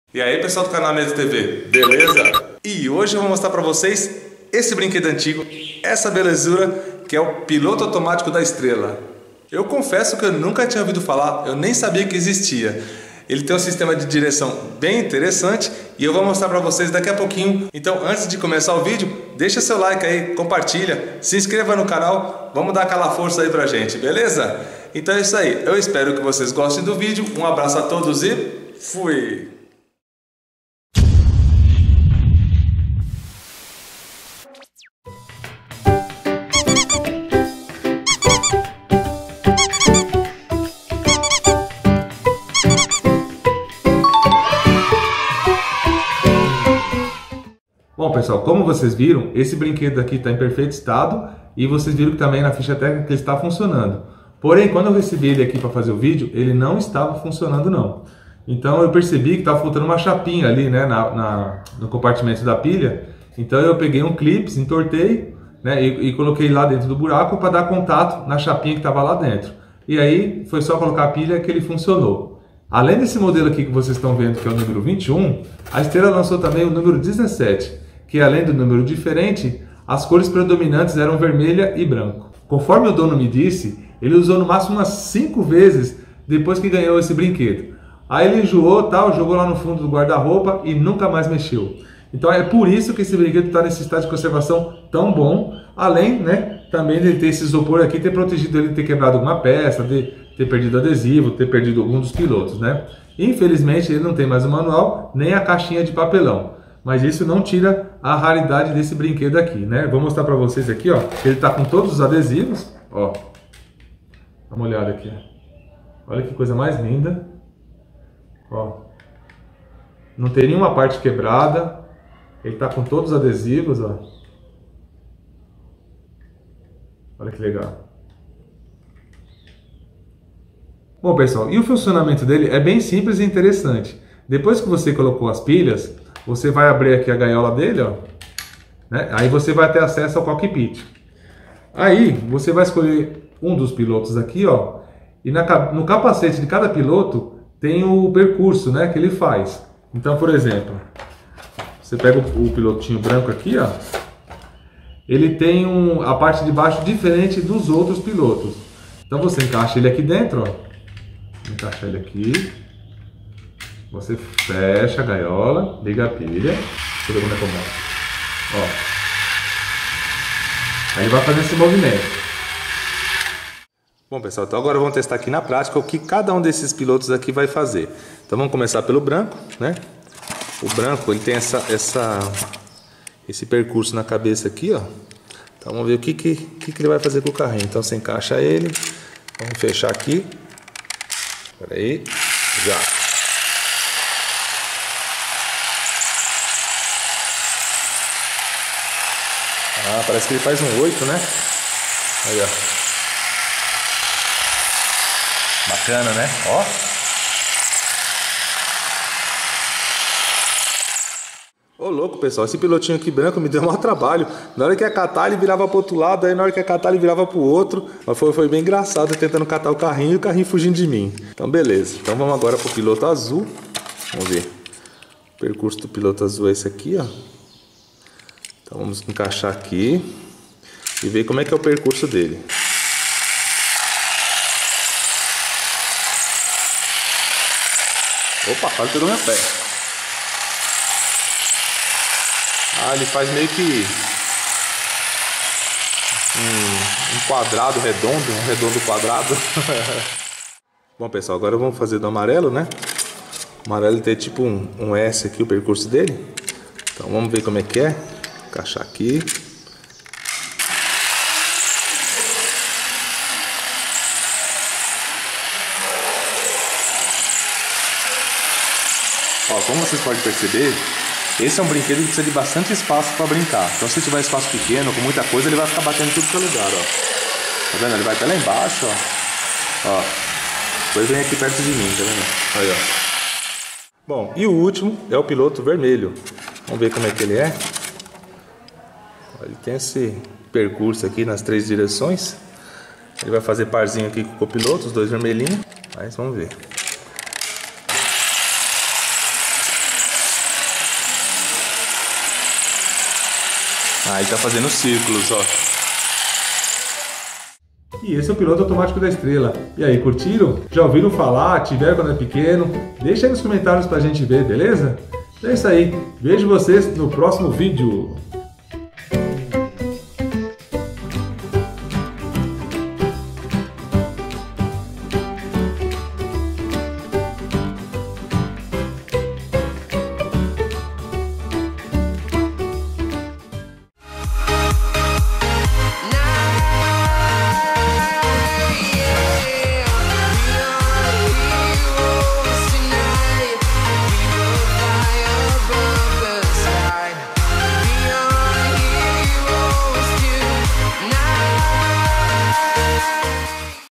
PILOTO AUTOMÁTICO DA BRINQUEDOS ESTRELA (BRINQUEDO ANTIGO / ANOS 80 / ANOS 90) - NIETTO TV MIX
Piloto Automático da brinquedos Estrela é um brinquedo bem interessante! Dependendo do piloto que você colocar dentro do carrinho, ele faz uma rota diferente.

Ele foi lançado nos anos 90 e este modelo do video está muito bem consevado.

Conheça mais sobre esse brinquedo assistindo o video!